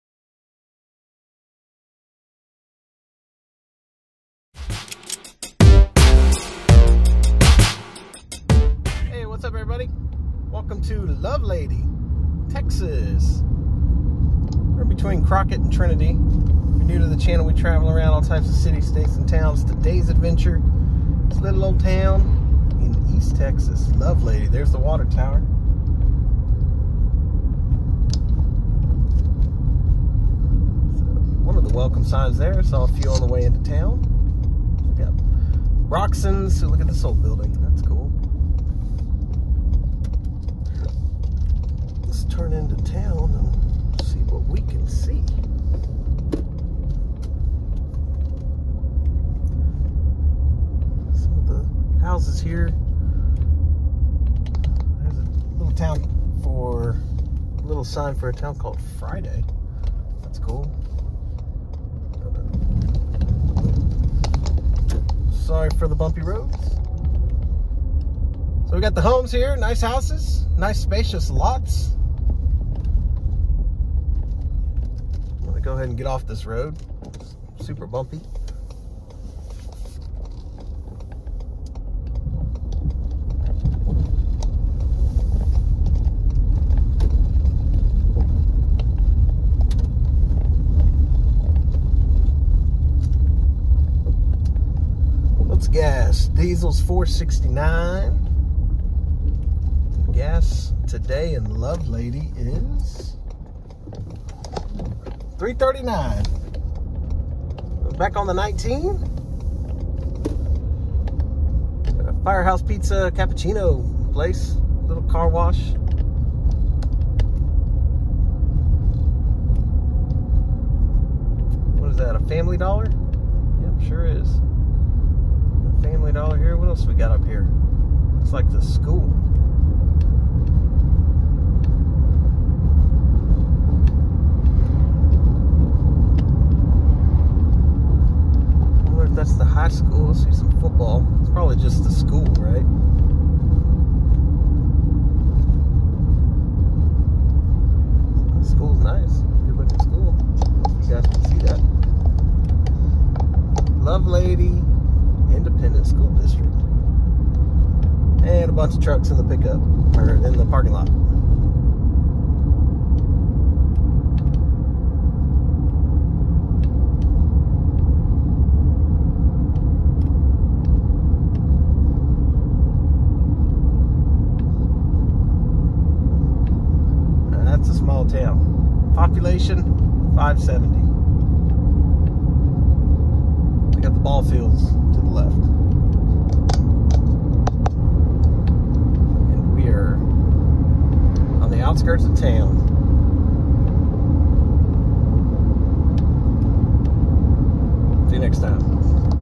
Lovelady, TX - Houston County - Small Town Living In East Texas
Lovelady is a town in Houston County, Texas, United States. The population was 570 at the 2020 census